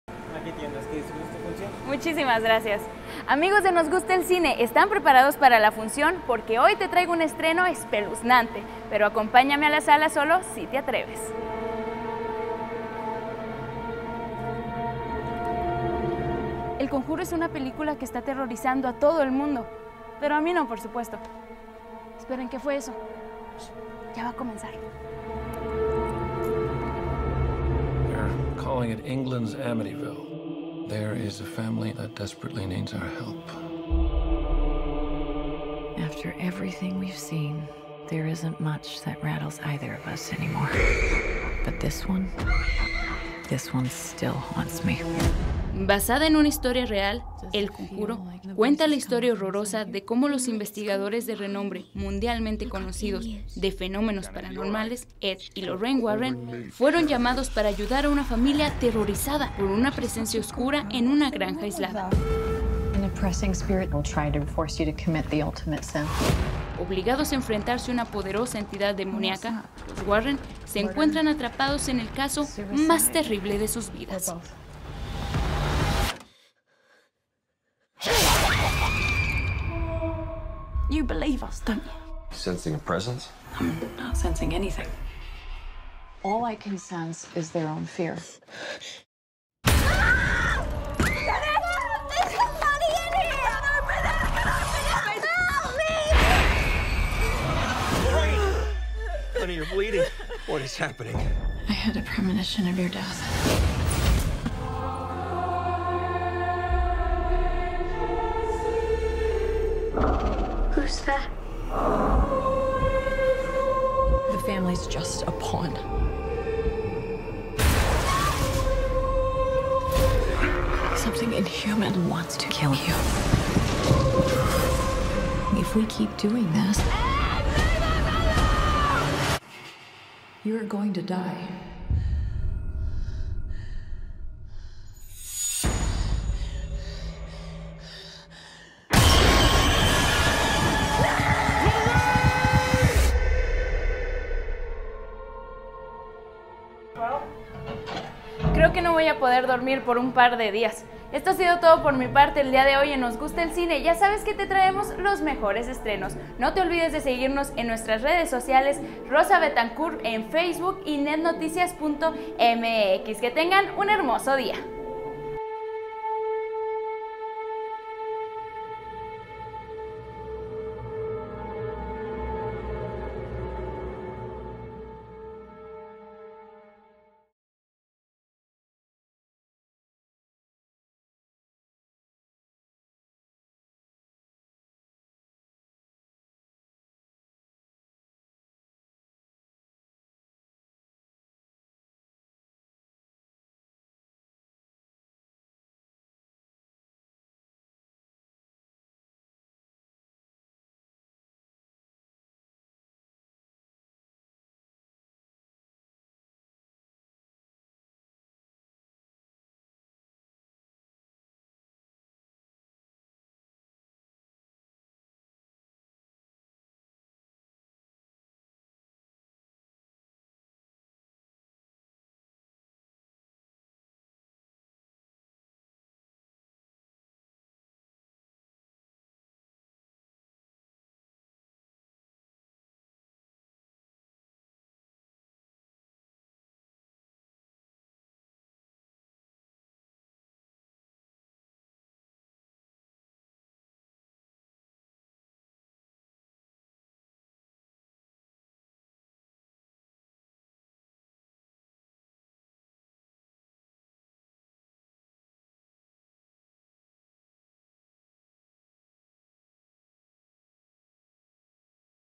Nos Gusta El Cine: El Conjuro 2
El Conjuro 2 ha sido una de las películas mas esperadas del verano. La segunda parte de la historia que aterrorizó al mundo entero es la número 1 de la taquilla.